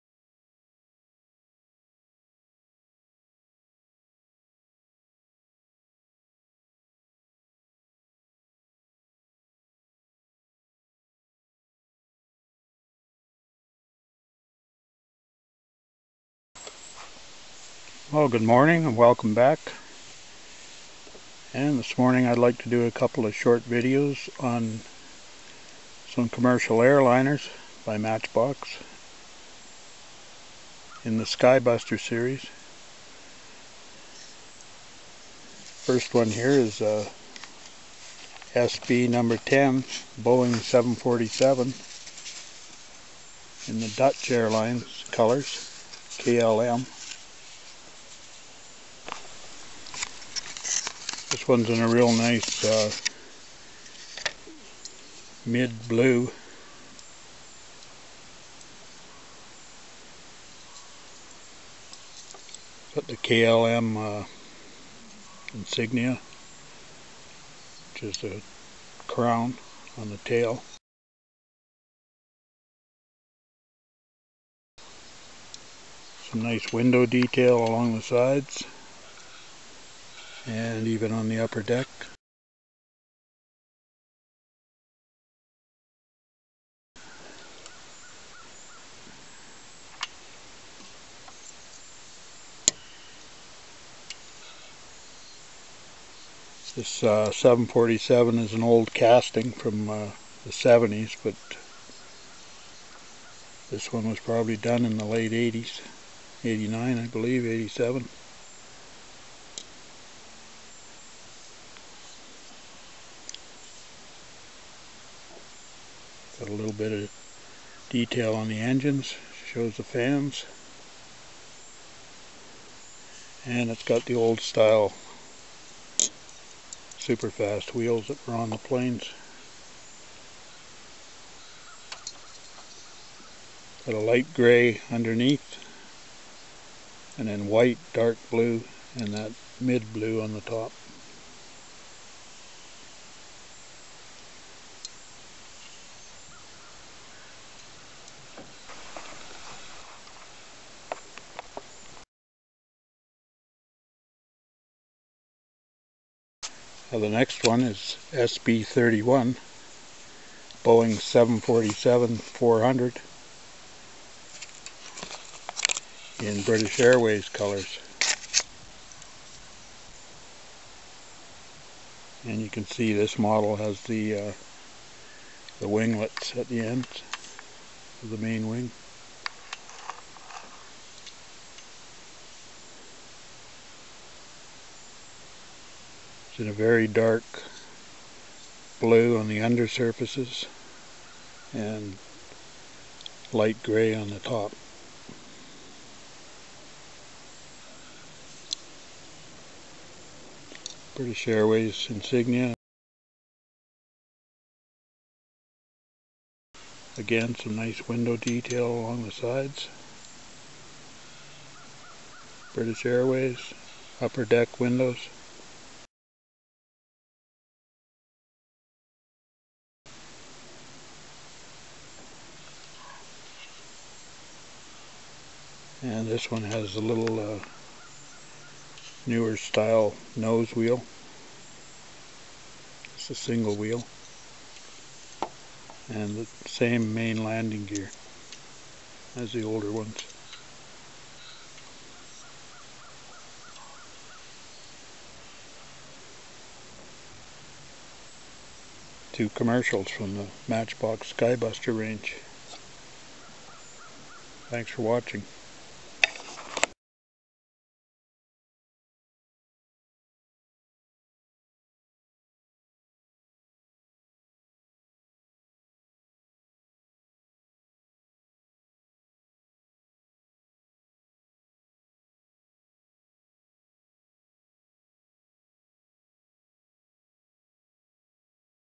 Matchbox Skybuster Boeing 747
Two of the commercial planes from Matchbox in the Skybuster Series.